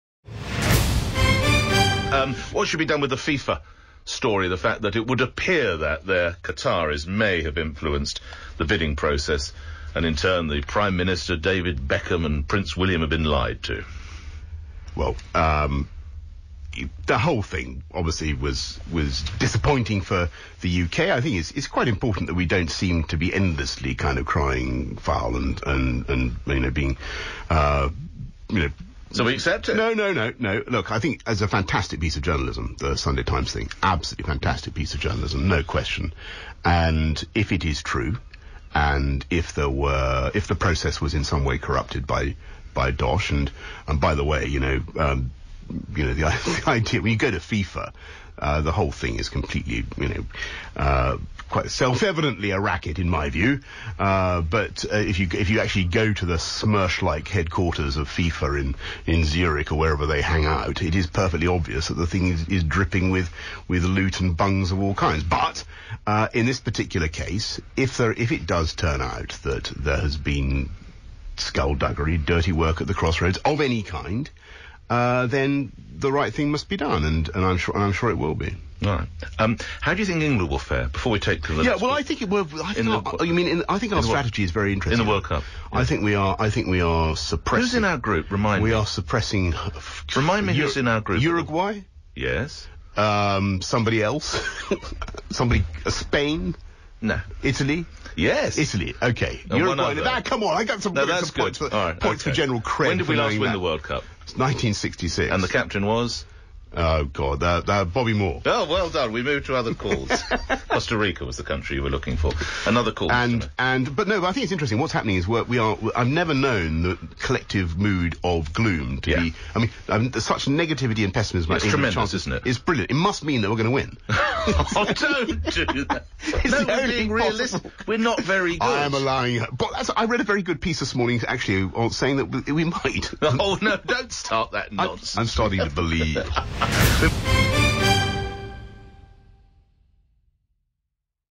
FIFA Full Of Bungs, Says Boris Over Qatar Allegations
Boris Johnson said Fifa is "dripping with loot and bungs" as he commented on the allegations over Qatar being awarded the 2022 World Cup.